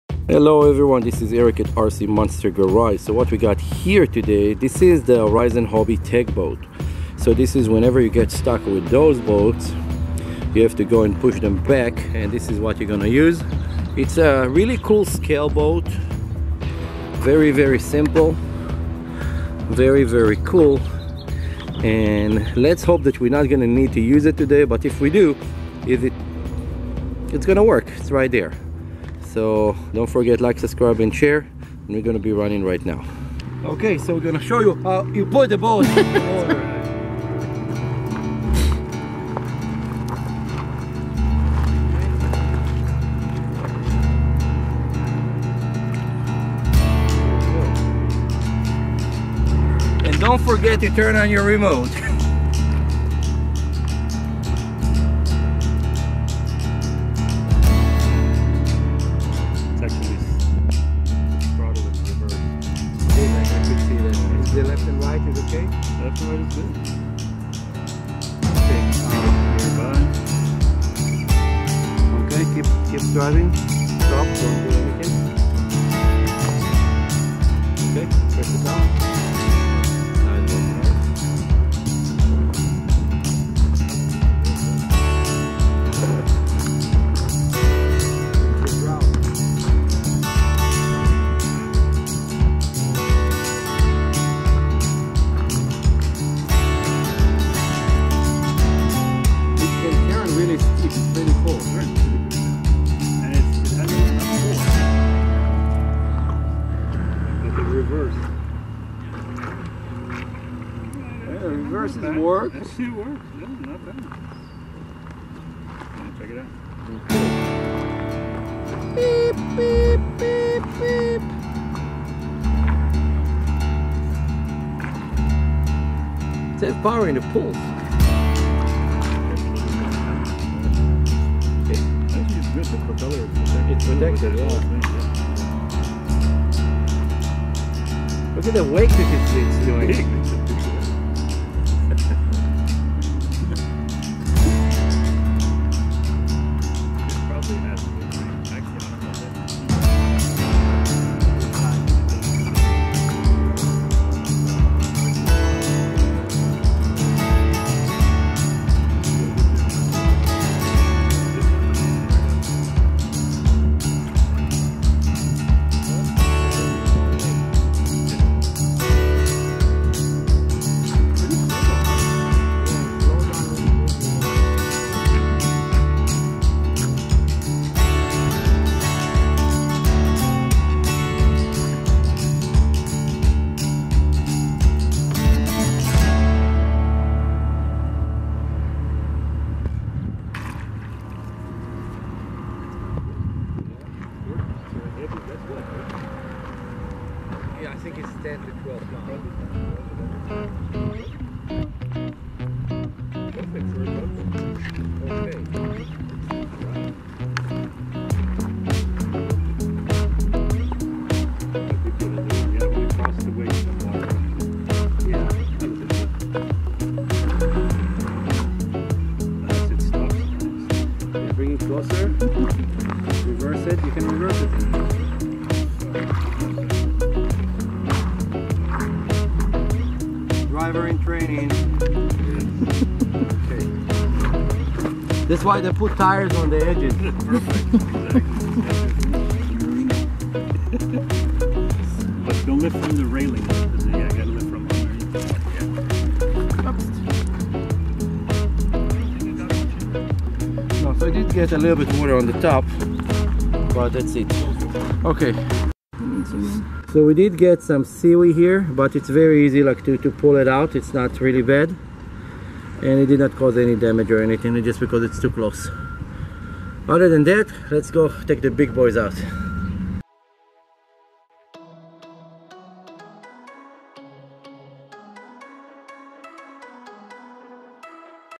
Horizon Harbor 30 Inch Tug Boat RTR
The Pro Boat® Horizon Harbor Tug Boat is a scale-appearing boat allowing you to kick back and be the captain of your own ship or create the ultimate retrieval boat.
Features
Durable, Molded Hull
Removable Retrieval Arms
Scale Detail
Operating Water Canon Ready
40 amp ESC
Torque Servo
2.4Ghz Transmitter
Boat Stand
700 Size Oversized Motor
Scale LED Lighting
Pre-Installed Ballast.

Approximate Assembly Time
No assembly required
Beam
8.75 in (222mm)
Completion Level
Ready-To-Run
Drive System
Propeller
Hull Height
10.5 in (266mm)
Hull Material
ABS
Motor Size
700
Product Length
30 in (762mm)
Product Weight
9.85lbs (4.5kg)
Speed Control
Included
Steering
Kort Nozzle